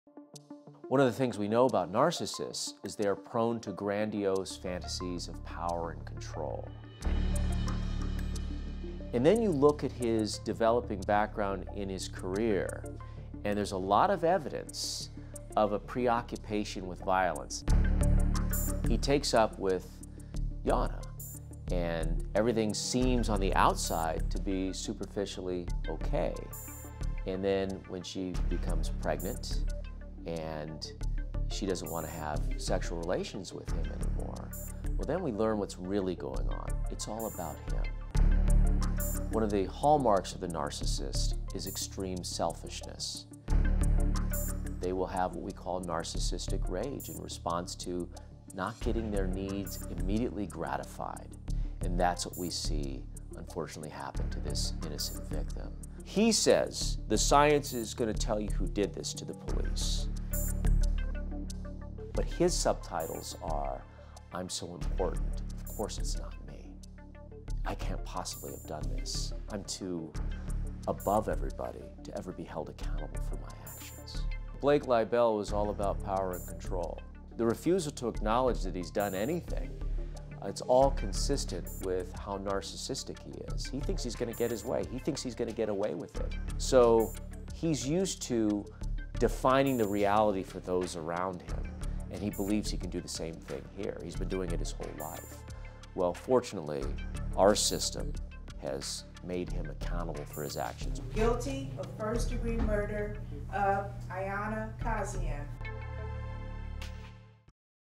Expert: "Blake Leibel was all about power and control"
Dr. Kris Mohandie is a forensic psychologist who was hired by the prosecution team in the event of a mental defect defense during Blake Leibel’s murder trial. Mohandie evaluates the actions that Blake took in murdering his girlfriend Iana Kasian, who had just given birth to their  daughter.